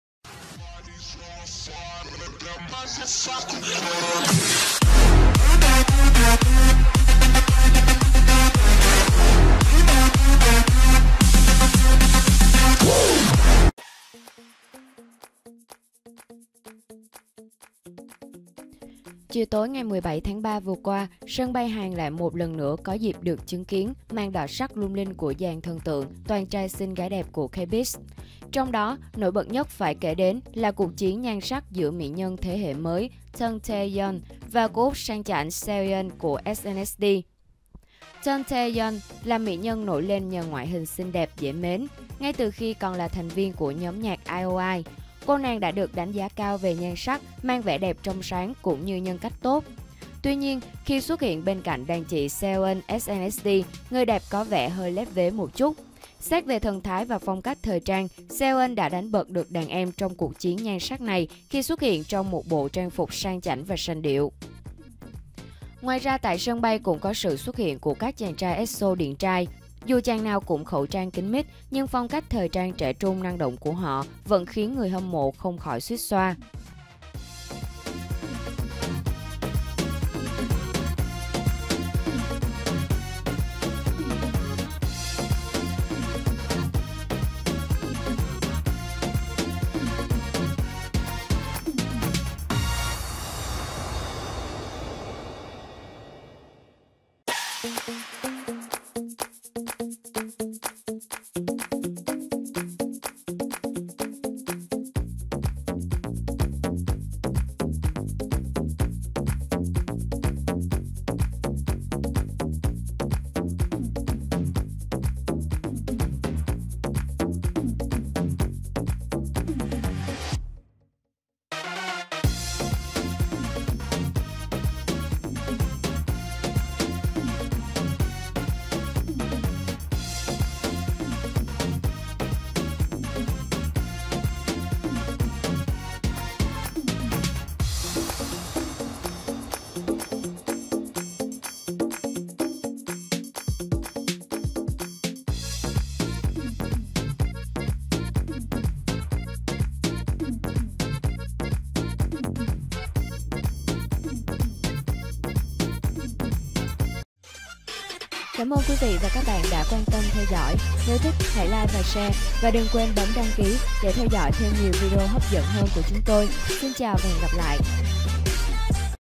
Seohyun “đánh bật” đàn em để trở thành mỹ nhân đẹp nhất tại sân bay - Tin tức của sao
Chiều ngày 17/3 vừa qua, một dàn thần tượng toàn “trai xinh gái đẹp” của showbiz Hàn đã có màn đọ sắc lung linh tại sân bay.